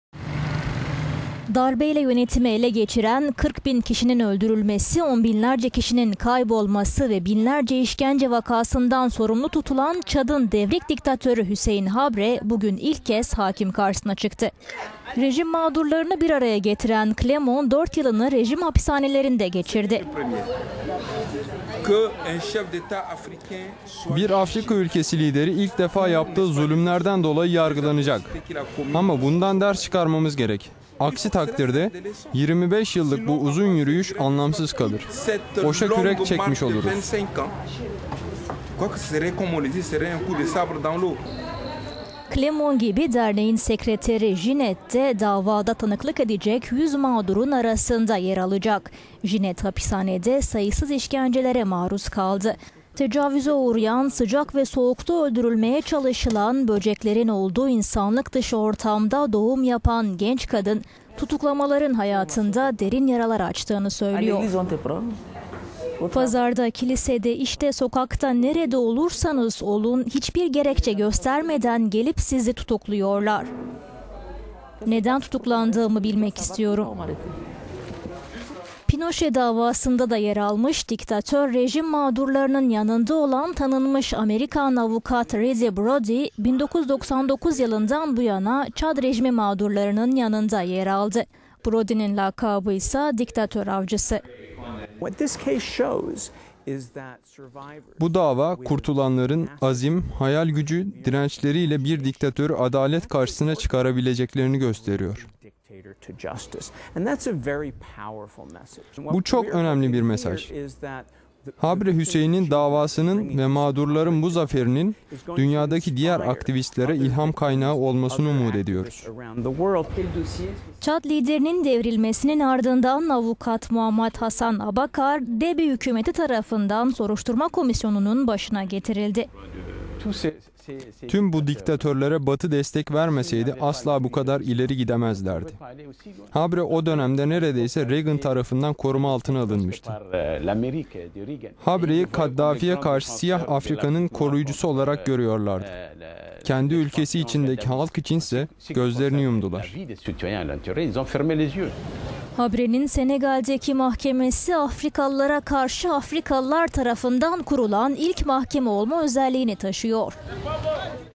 Çad'ın devrik diktatörü Habre Senagal'de hakim karşısında
Çad'ın devrikdiktatörü Hissein Habre rejiminin mağdurları kurduğu dernek sayesinde istediklerini elde etmeyi başardı. Darbeyle yönetimi ele geçiren, 40 bin kişinin öldürülmesi, on binlerce kişinin kaybolması ve binlerce işkence vakasından sorumlu tutulan Habre 20 Temmuz 2015'te ilk kez hakim karşısına çıktı.

25 yıl süren işkenceden kurtulan rejim mağdurları 15 yıl önce ilk şikayetlerini resmi olarak yetkili makamlara sundu. Beklenen mahkeme nihayet gerçekleşti.